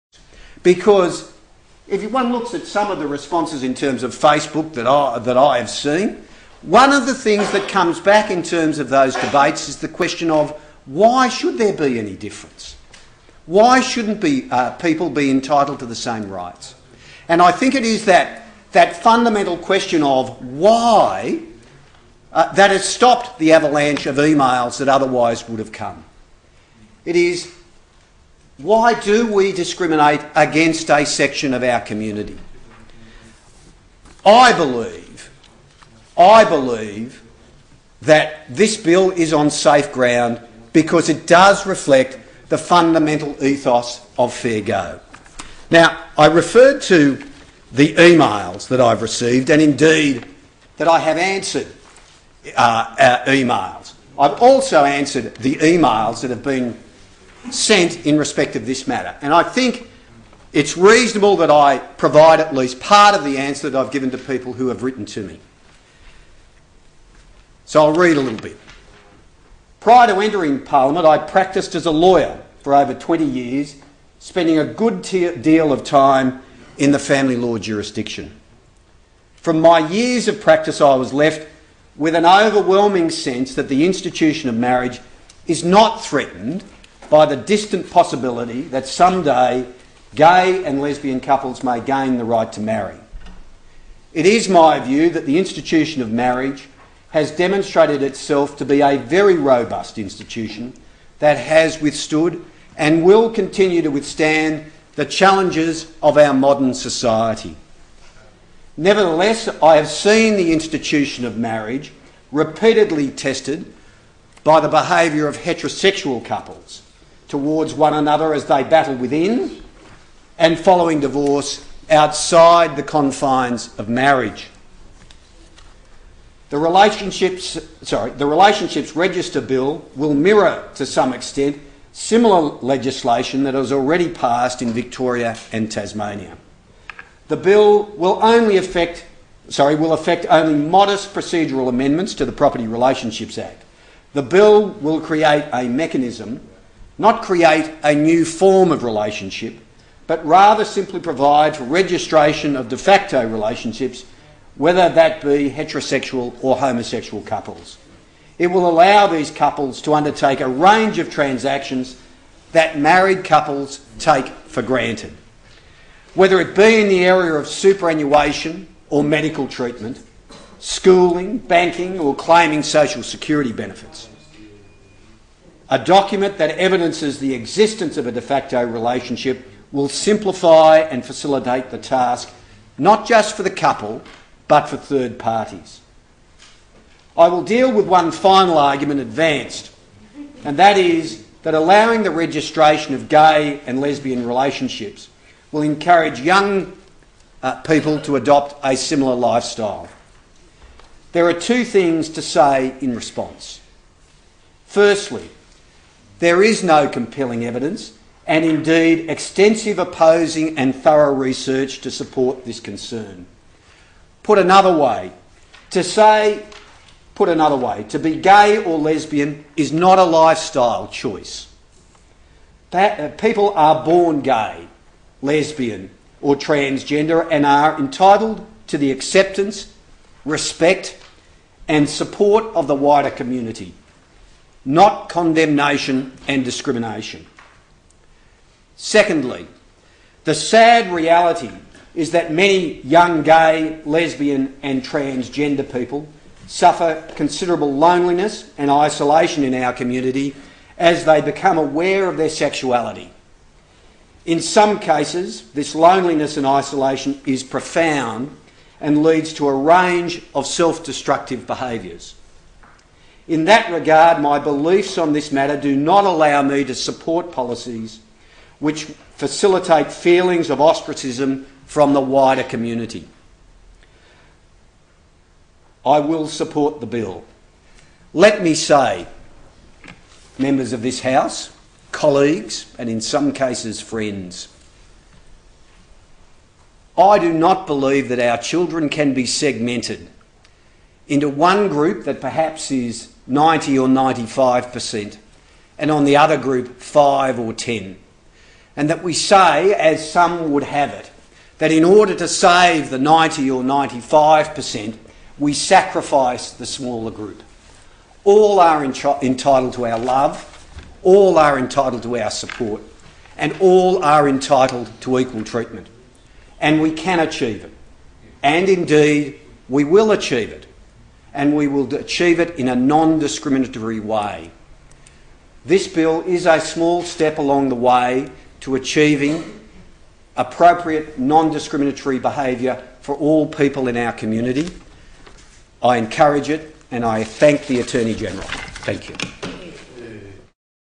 Khan speaks to Relationships Register Bill Part 2
Part 2 of Trevor Khan's contribution on the Relationships Register Bill NSW 2010